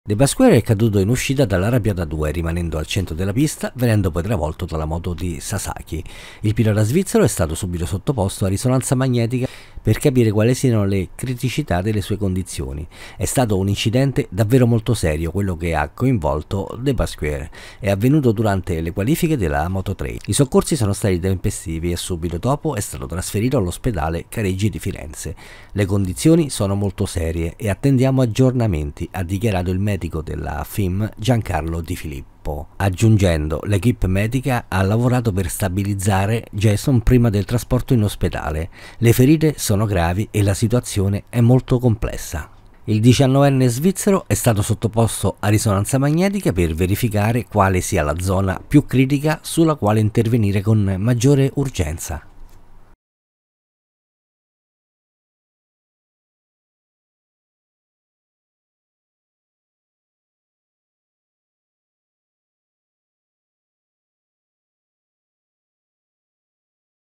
Dupasquier non ce l'ha fatta, è morto a soli 19 anni. @VideomakerProSport​
Dupasquier non ce l'ha fatta, è morto a soli 19 anni.
Dupasquier dopo l'incidente era stato trasportato d'urgenza all'ospedale di Careggi a Firenze dove erano stati evidenziati numerosi danni e condizioni gravissime l'intervento chirurgico subito stanotte non è servito a salvarlo e poco fa è arrivato il triste annuncio.
La redazione di videomakerpro.it  si unisce al dolore dei famigliari, della scuderia, dei colleghi e degli amici tutti.
Un incidente multiplo ha coinvolto l'elvetico, Ayumu Sasaki e Jeremy Alcoba. Dupasquier, pilota della KTM, è rimasto a terra sull'asfalto ed è stato trasportato in elicottero presso l’ospedale Careggi di Firenze.
Le sue condizioni sono subito apparse molto gravi, il pilota è stato sottoposto a risonanza magnetica per poter capire dove il personale medico dovrà intervenire più in fretta, nella notte è stato operato, ma non ce l'ha fatta, troppo gravi le ferite riportate nella caduta.
Il medico della FIM Giancarlo Di Filippo aveva commentato preannunciando la gravità della situazione così: "L'équipe medica ha lavorato per stabilizzare Jason prima del trasporto in ospedale. Le ferite sono gravi e la situazione è complessa".